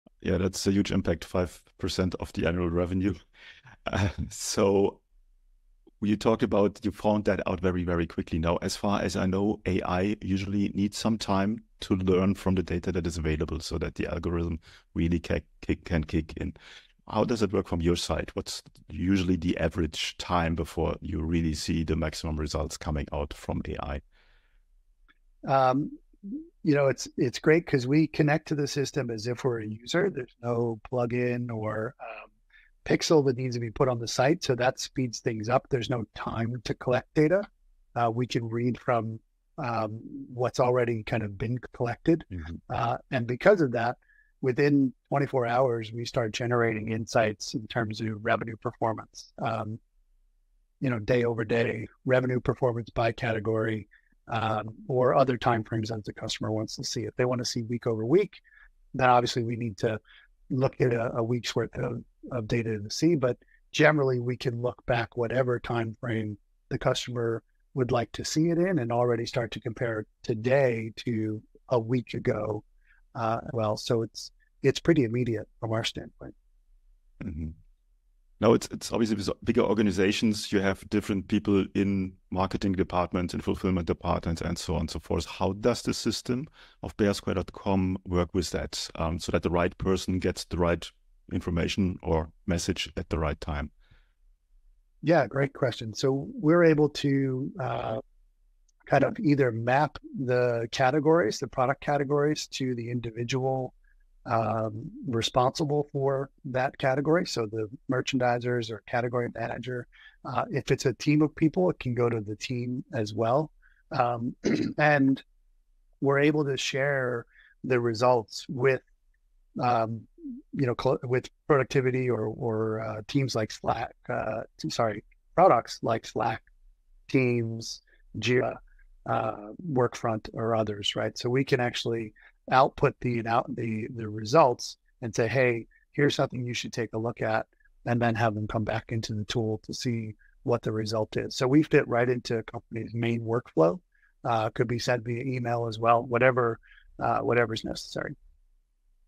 How To Boost Ecommerce Revenue with AI
🚀 Excited to share insights from the latest episode of the Ecommerce Coffee Break podcast! Join us as we dive into the world of AI-powered revenue growth in ecommerce with Bare Square's Head of Revenue, Brian White. Discover how AI is transforming marketing strategies and optimizing revenue streams. Don't miss out! AI RevenueGrowth 📈☕️

Tune in: t.ly/nAcN6